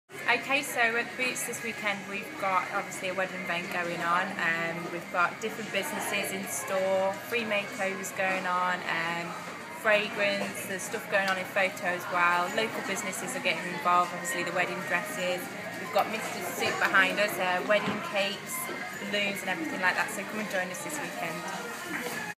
Boots' Wedding Promotion
Laura Jane Vernon talks about the wedding promotion in Stafford's Market Square  Boots on 11th, 12th and 13th May 2012.